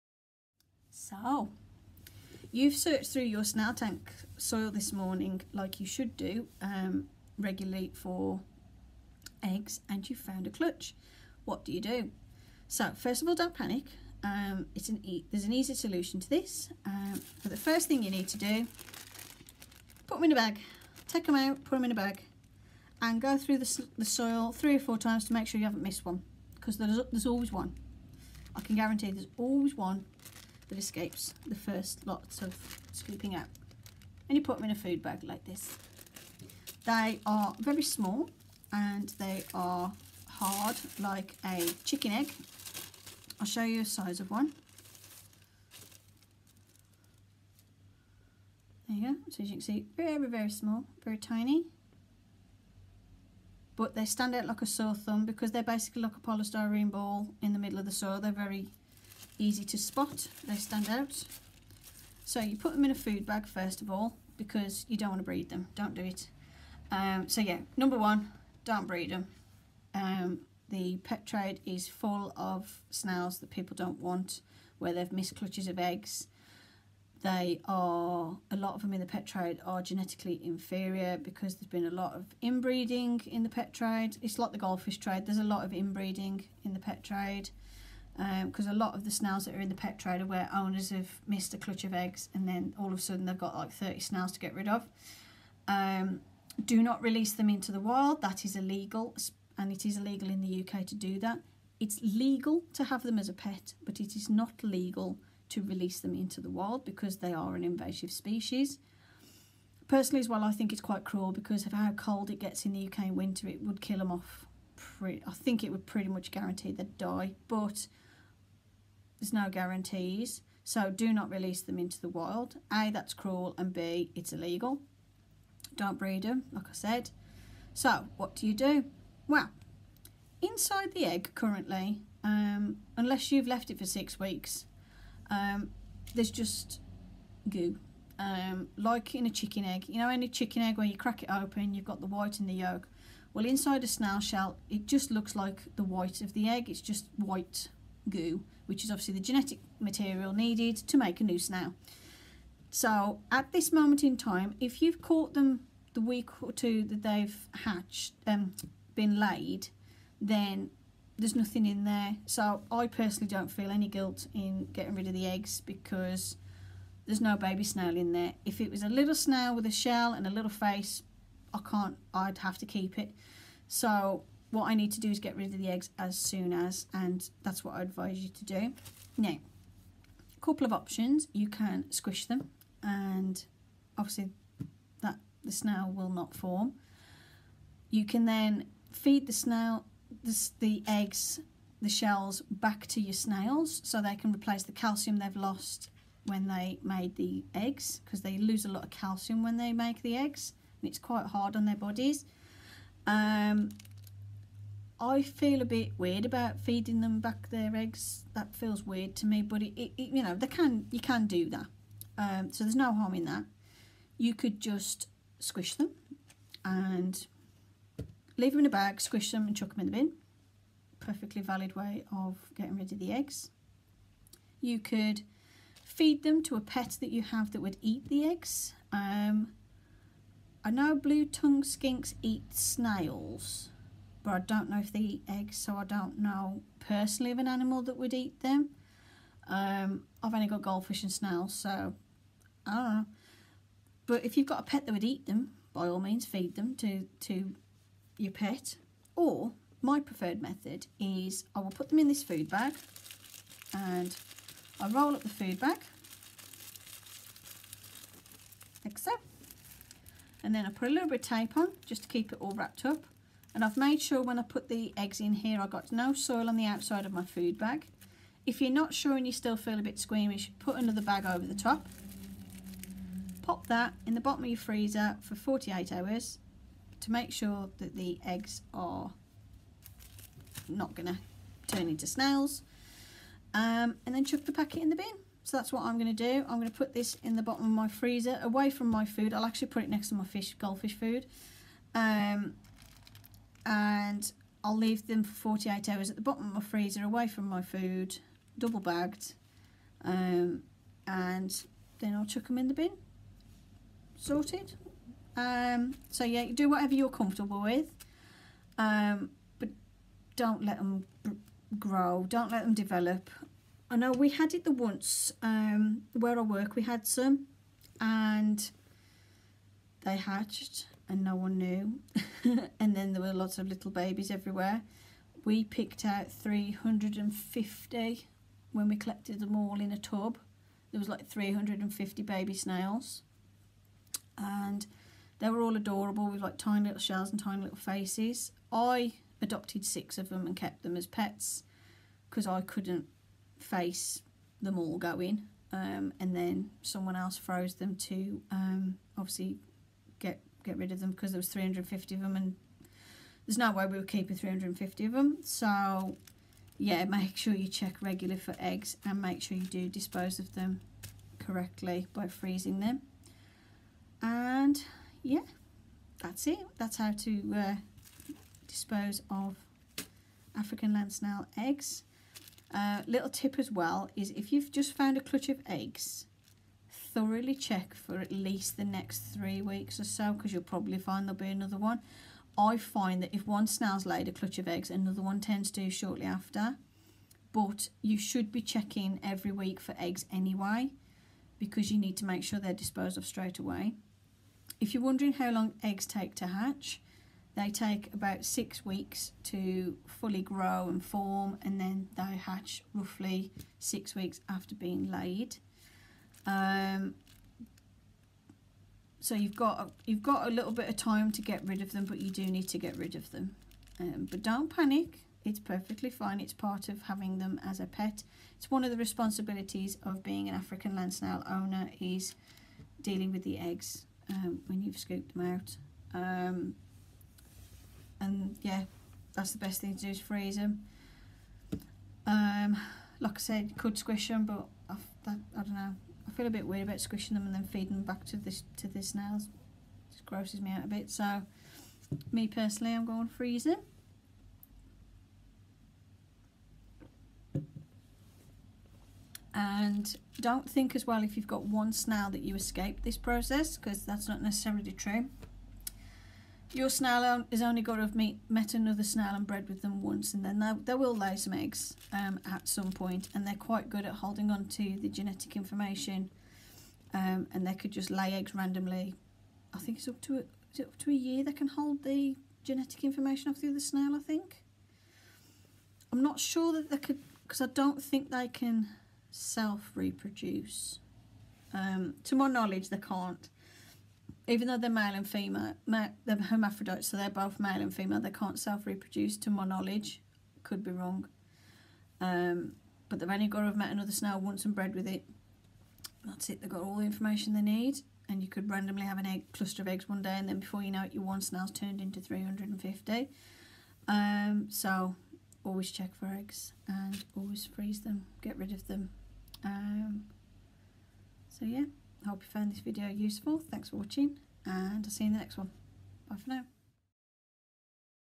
Snail eggs? Don’t panic!
What to do if your giant African land snails lay eggs and how to dispose of them.